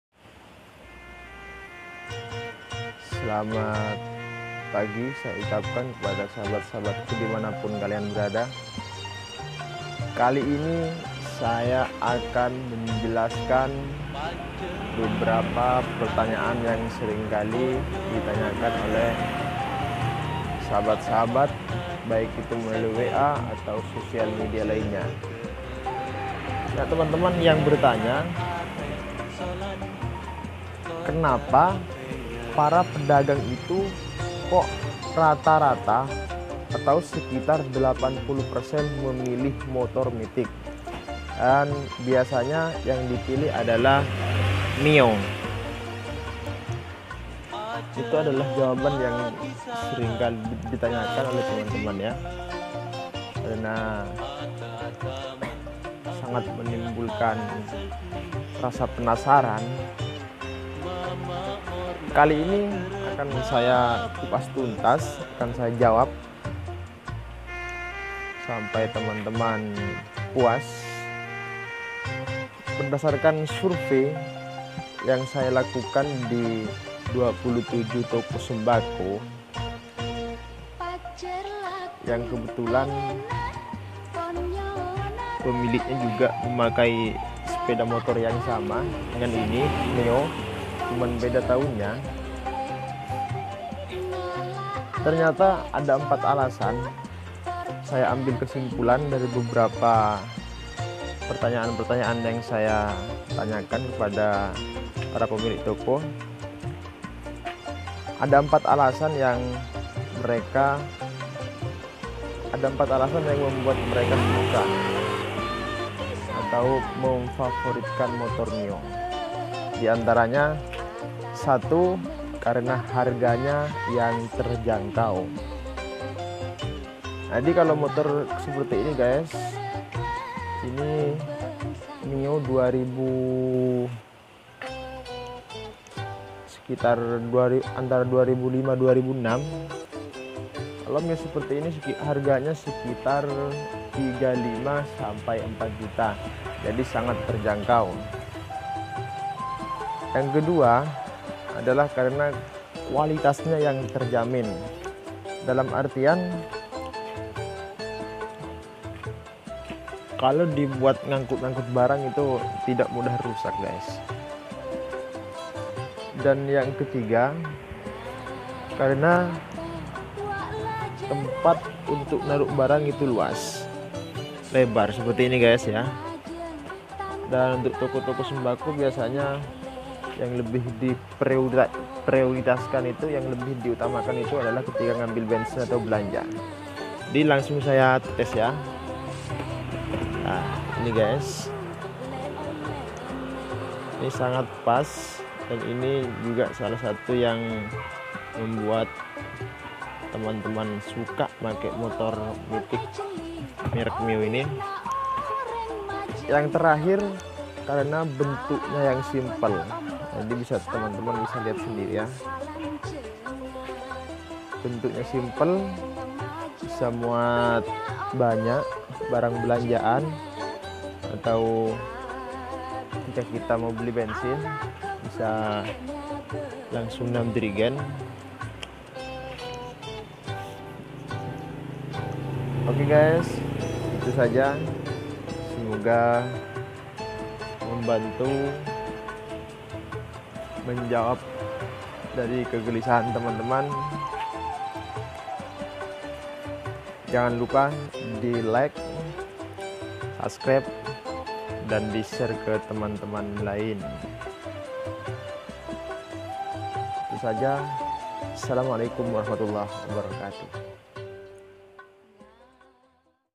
Ternyata, INI ALASAN DI BALIK BOOMINGNYA MIO LAWAS
Pengusaha Sembako, mayoritas memilih Sepeda Motor MIO sebagai alat transportasi. di Balik boomingnya motor tersebut di perkirakan sekitar 80%. WOW!

Ternyata ada 4 alasan. Silahkan tonton sampai selesai. Terima kasih